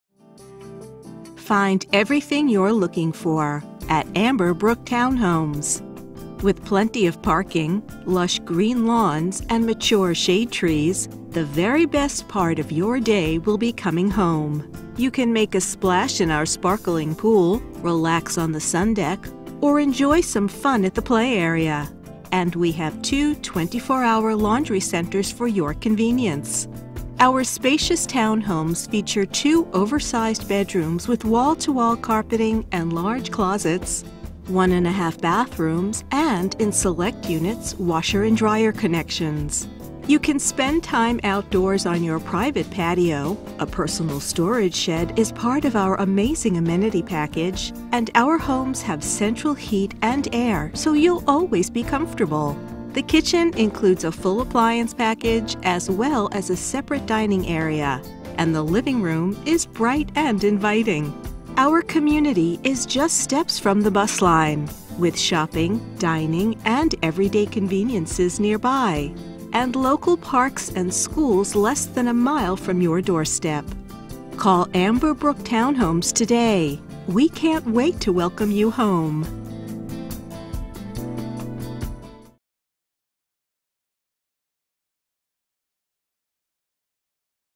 Amberbrook Apartments in Kennewick, WA - ForRent.com
We've got amazing two bedroom one and half bath townhomes in a convenient Kennewick location close to shopping, restaurants and entertainment. All homes include fully equipped kitchens, central heat and air conditioning, many with full size washer/dryer hookups and personal patios with storage shed and two free assigned parking spaces. Our community offers two seasonal swimming pools, two onsite 24 hour laundry centers, a play area. Amberbrook Townhomes is within walking distance to bus line, schools and shopping. A member of the Crime Resistant Community Living program of Kennewick. - 4321 W Hood Ave A-12